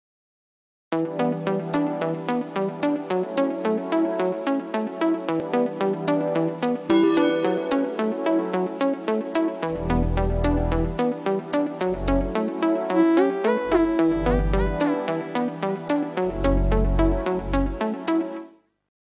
RPReplay Final1581220315
Augmented reality mural animation located in Fawkner Melbourne for City of Moreland